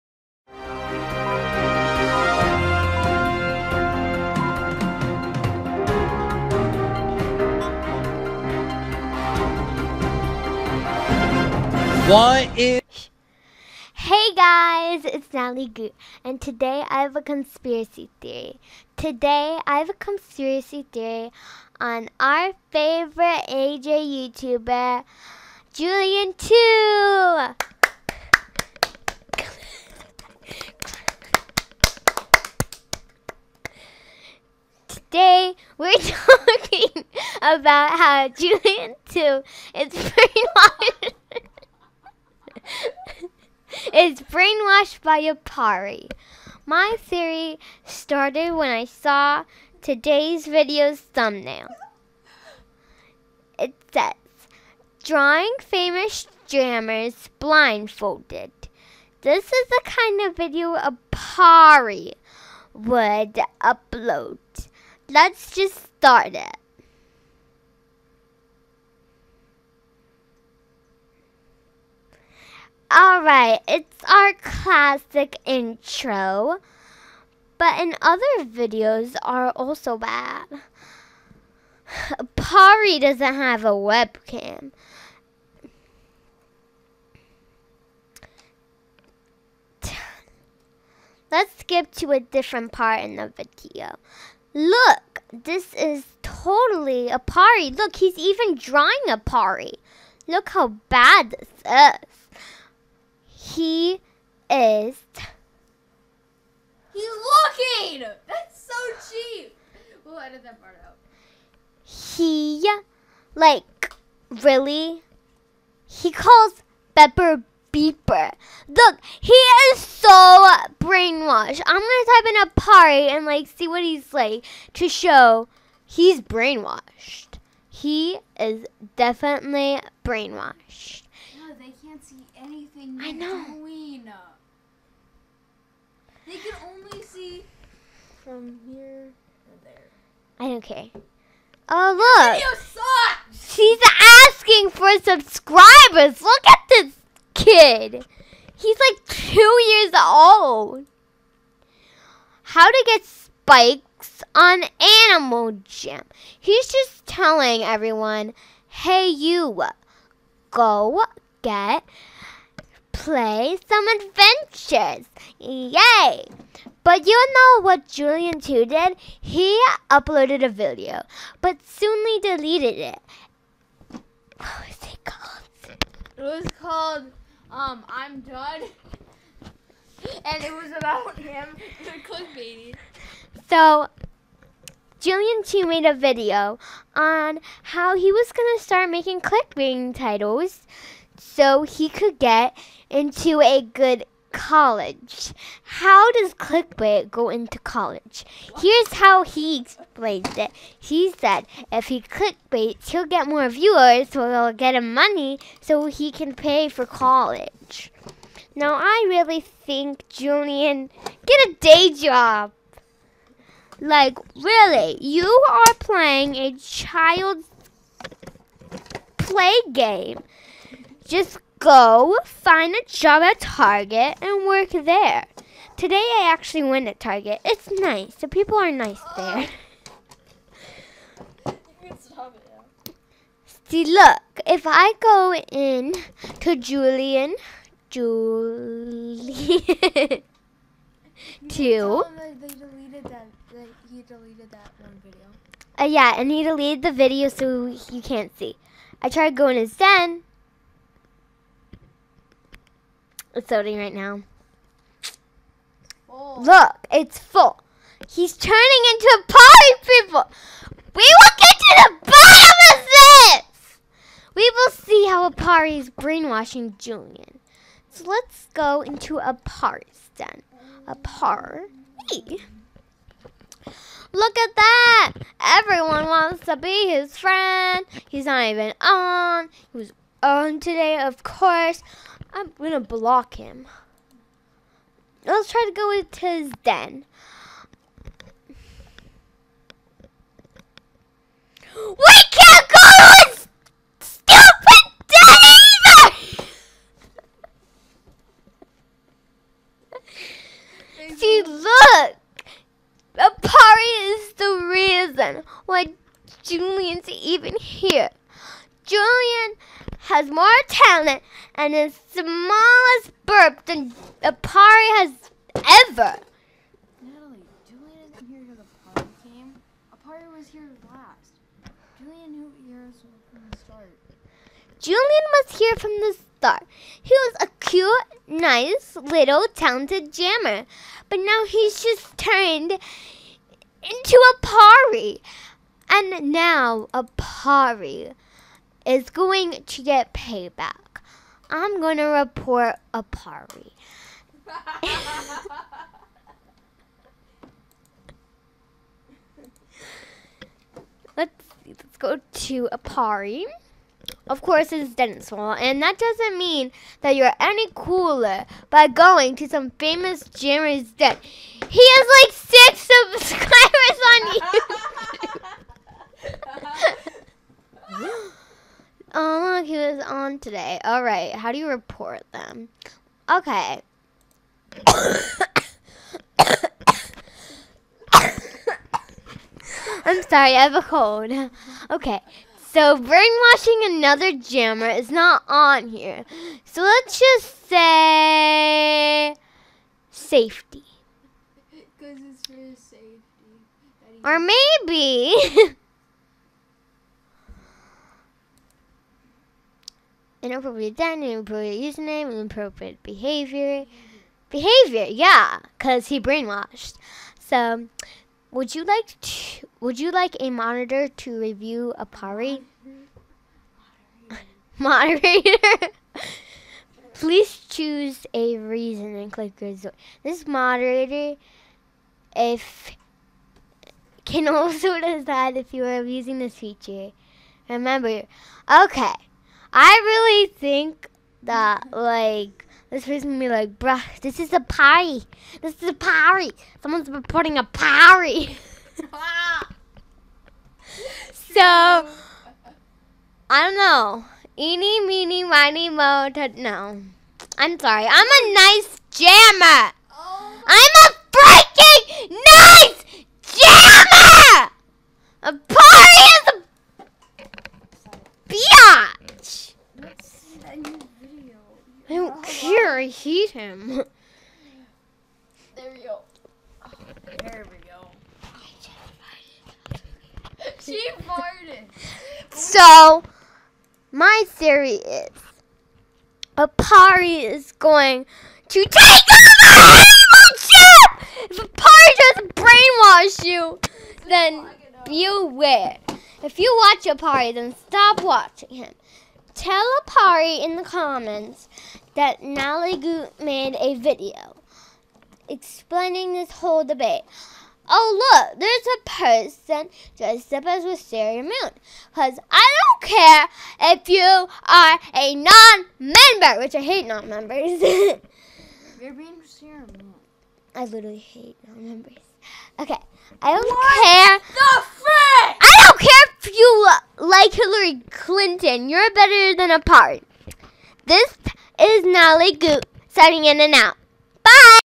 Animal Jam Conspiracy Theory | Julian2 Brainwashed
Hello everybody, today I am going to talk about a conspiracy theory on Julian2 being brainwashed by the worst 2 year old in the world, Aparri!!

Toodles!!

(DESCRIPTION WRITTEN BY DAVYMAK AJ)